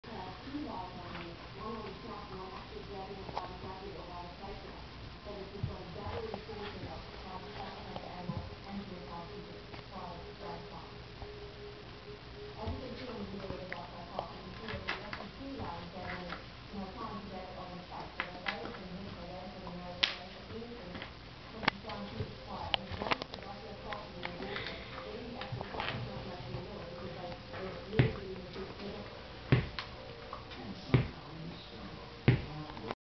Puppy
griggeo playing with an ice cube